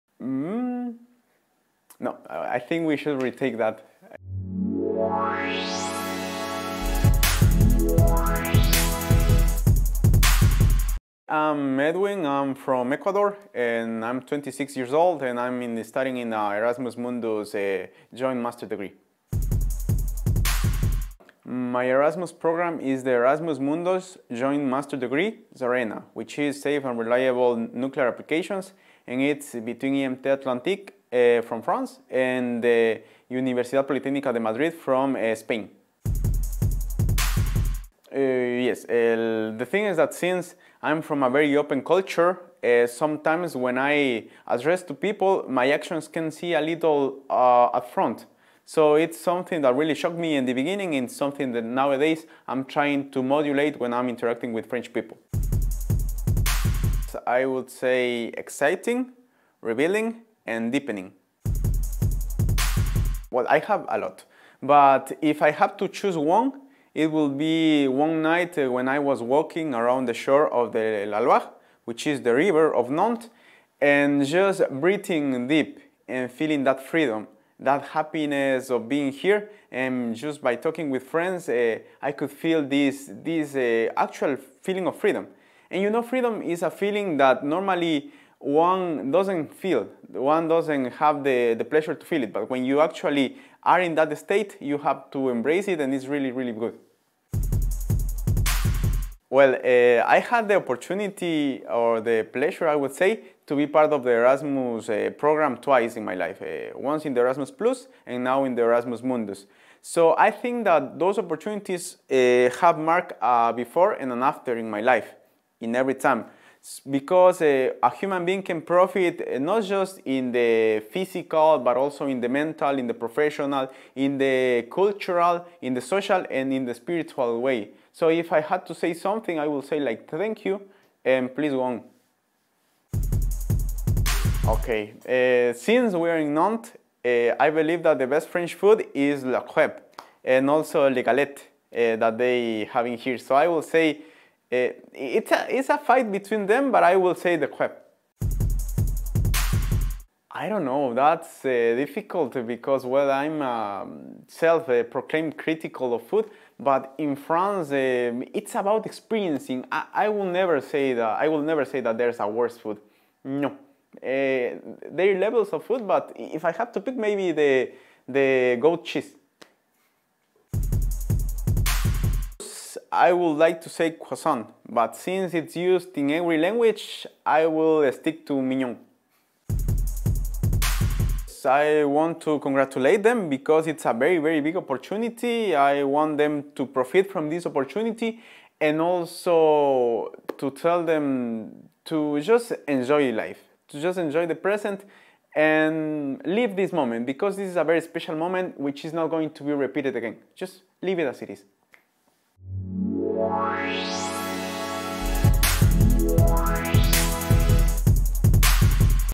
Erasmus+ : Interview d'Edwin Córdova
Interview d'Edwin Córdova, un étudiant équatorien de l'Erasmus Mundus Joint Master Degree SARENA.